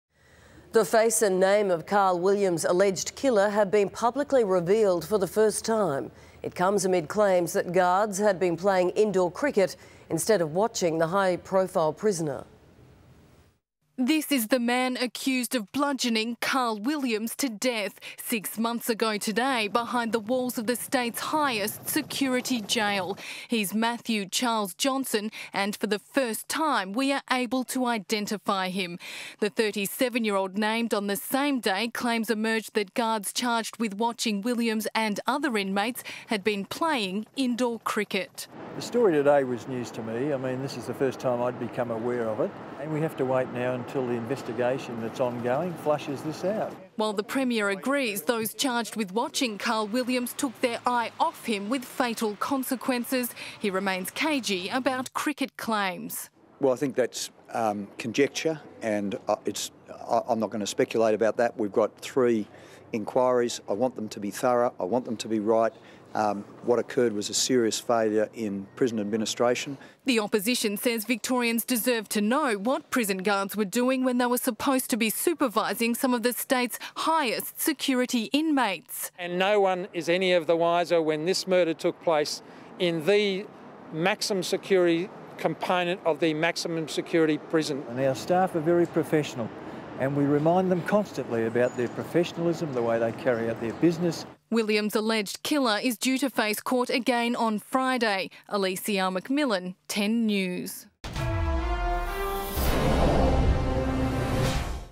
Williams' Killer Revealed
The face and name of Carl Williams' alleged killer has been publicly revealed for the first time.